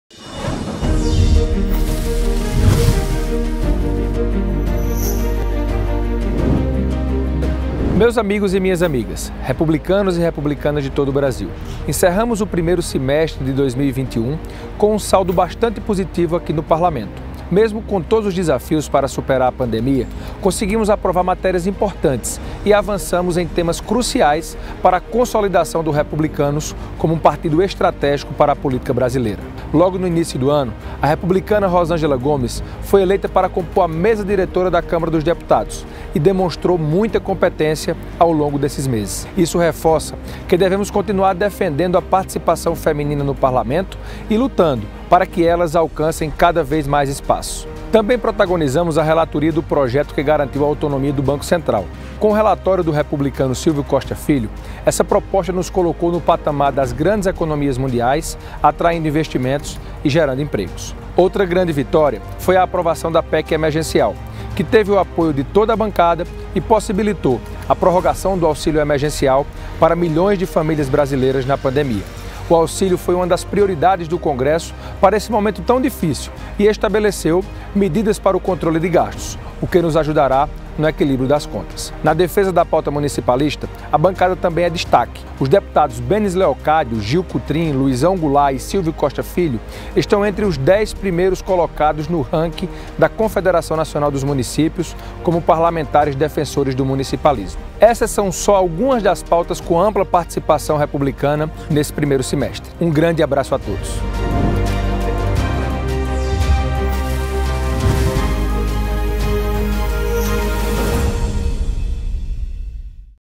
Balanço do 1º semestre: Republicanos protagonizou a aprovação de importantes matérias - Parte 1
Deputados foram destaque na defesa de propostas, como a da autonomia do Banco Central e a da PEC Emergencial, que possibilitou a prorrogação do auxílio.

Dê o play e confira.